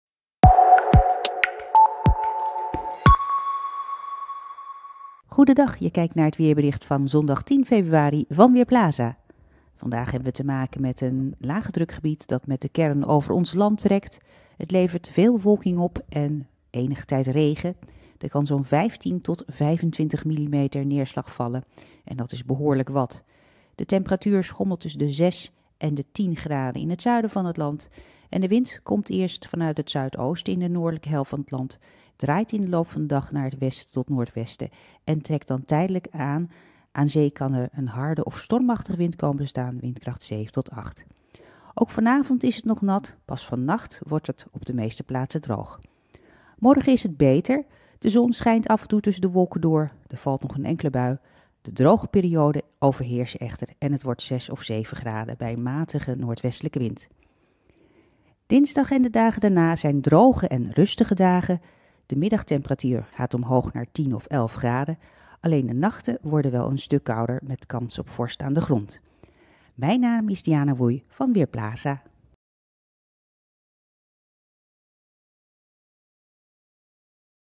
Weerbericht voor zondag: Derde natte dag van het jaar
Het is een natte zondag. Er wordt 15 tot 25 mm regen verwacht. Deze hoeveelheid levert flinke regenplassen op. Het regent ook enige tijdlang. Normaal gesproken komen in een jaar gemiddeld 22 natte dagen (binnen een etmaal 10 mm neerslag of meer) voor. Vorig jaar zijn er slechts 14 natte dagen geteld.

Vandaag is de derde natte dag van het jaar 2019. In Januari viel over het algemeen minder neerslag dan normaal, maar leverde wel een natte dag op. Februari heeft tot nu toe al 32 mm regen gebracht, terwijl er in een doorsnee februarimaand 55 mm neerslag wordt geregistreerd. Op 6 februari is in De Bilt 15 mm regen gemeten.